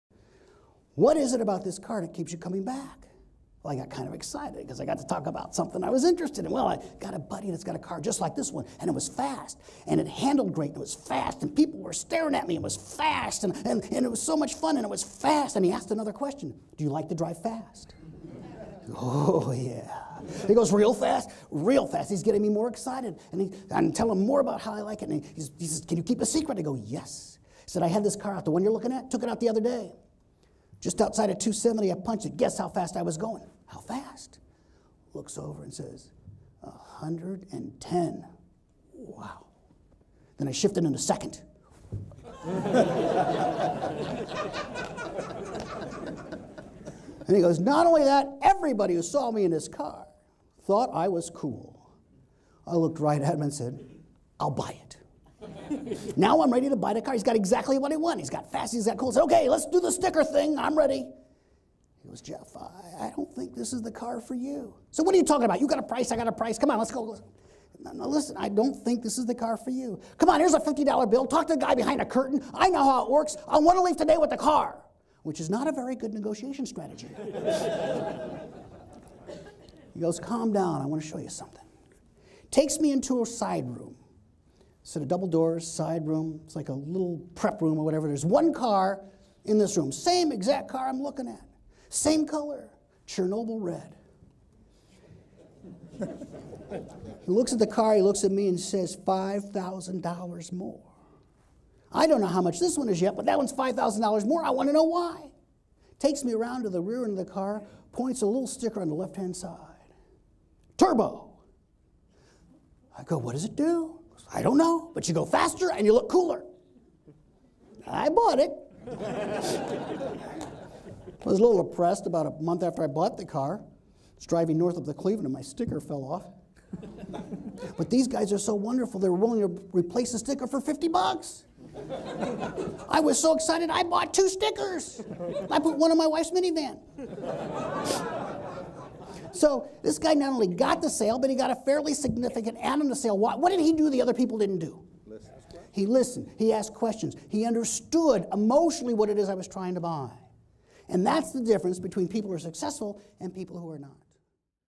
Finding The Hot Buttons: From "Selling Made Super Simple" Keynote
In this clip, Jeff Slutsky's audience learns that a big key to success is effective communication techniques.  Asking the right kinds of questions and listening are two of the most important approaches to influencing the people around you.  In this segment, Jeff shares a funny story about buying a car that the audience easily identifies with.